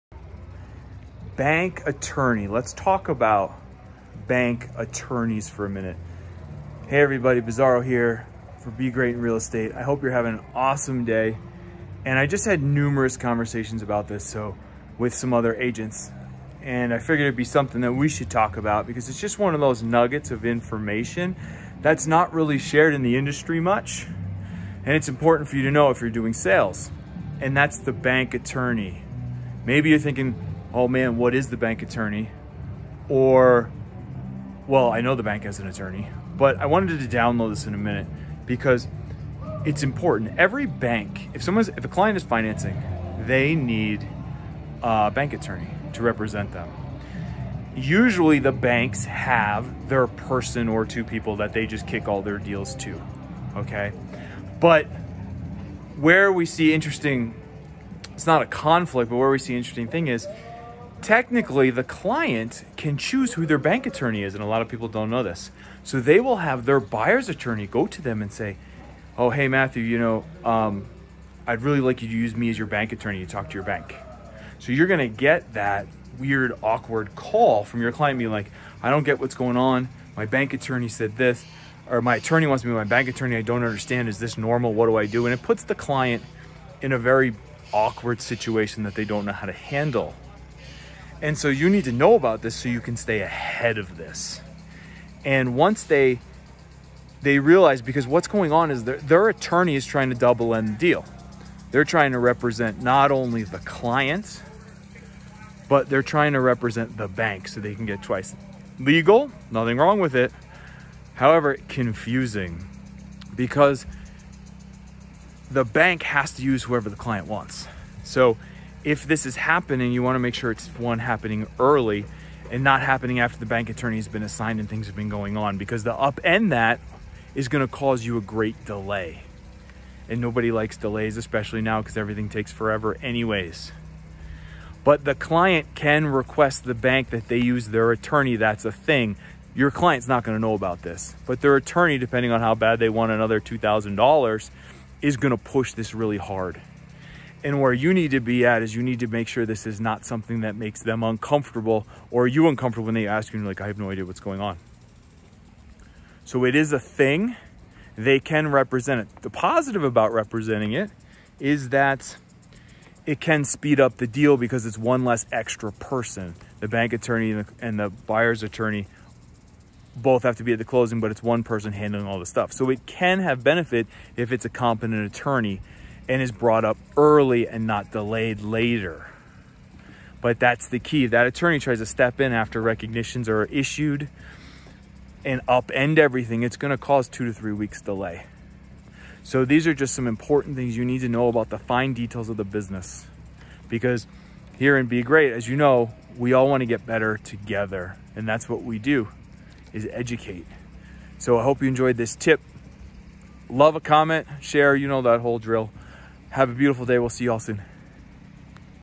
What happens if you get a weird call from your client...
…and they say “My attorney wants to be my bank attorney. I don’t understand. Is this normal? What should I do?”.